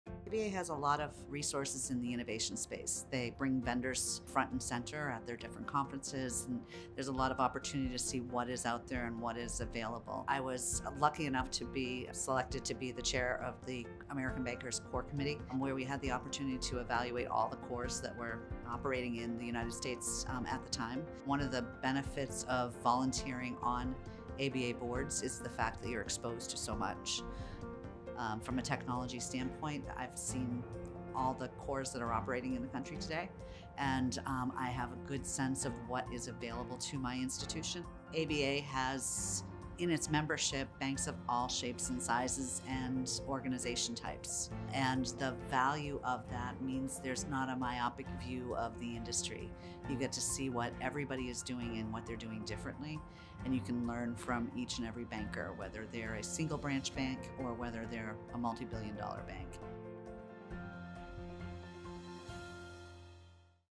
The Value of ABA Membership: Julieann Thurlow Testimonial
Julieann Thurlow, President & CEO, Reading Cooperative Bank, shares how ABA membership provides opportunities for banks of all sizes to innovate and stay on the cutting edge of technology.